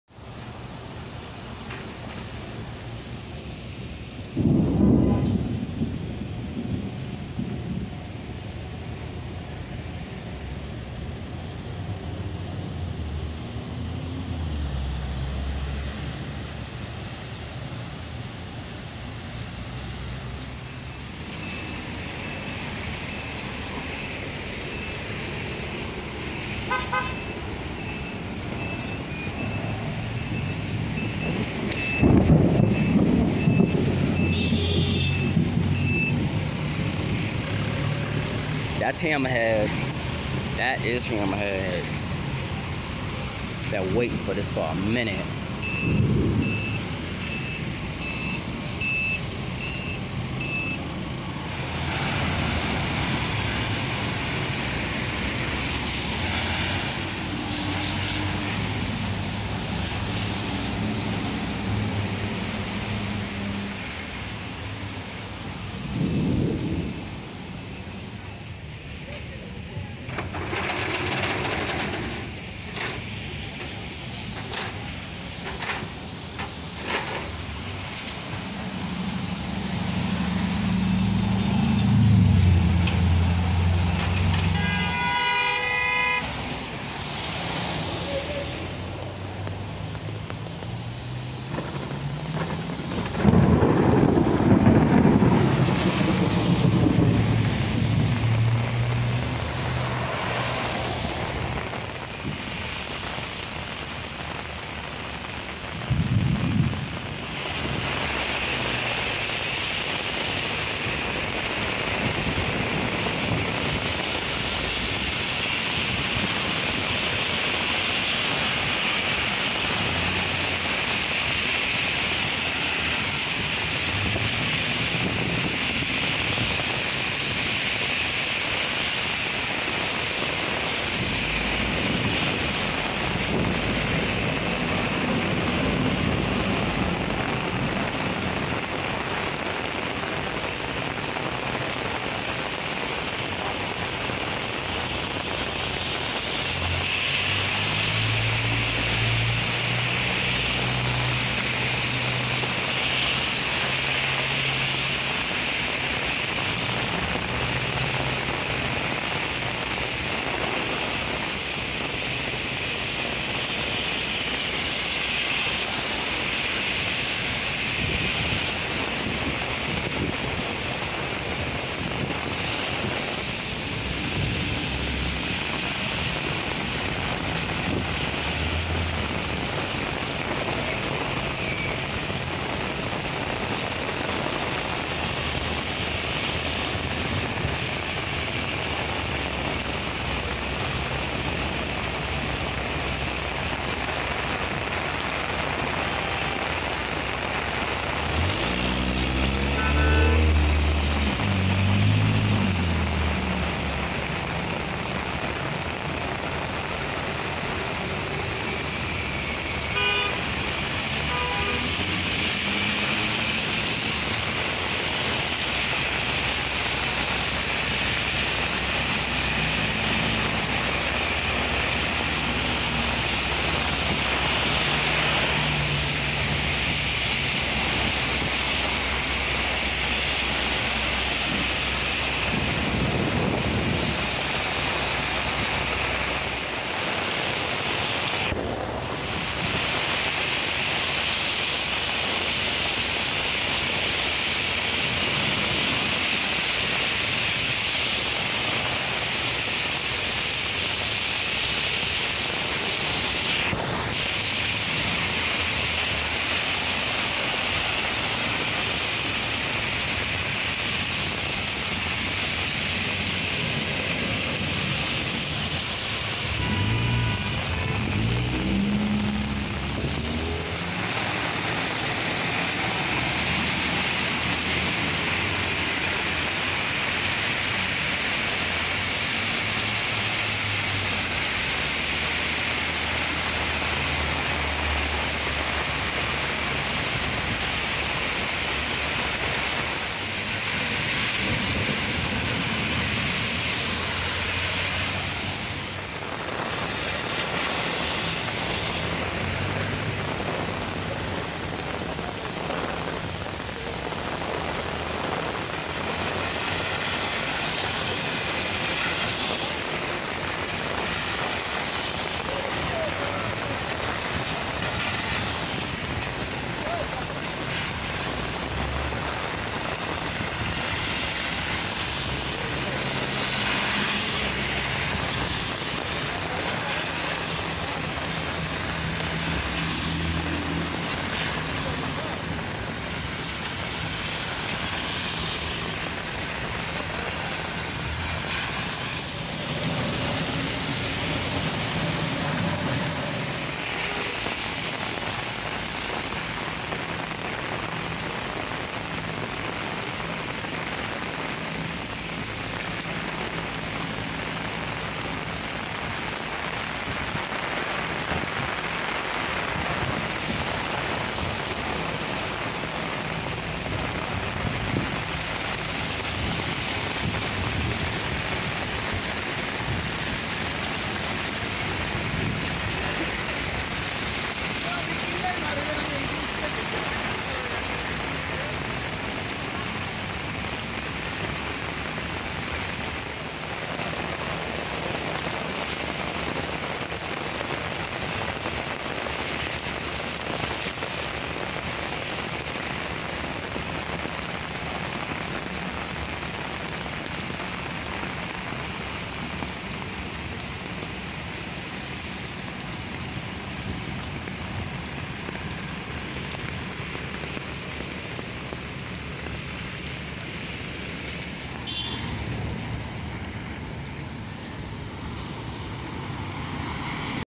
Massive Thunderstorm slams Kings(Brooklyn) New York 7/25/22
My footage of a massive Thunderstorm that produced quite the amount of Heavy Rain, Rumbles of thunder and some wind!

This was recorded out in Cypress Hills/ East New York Brooklyn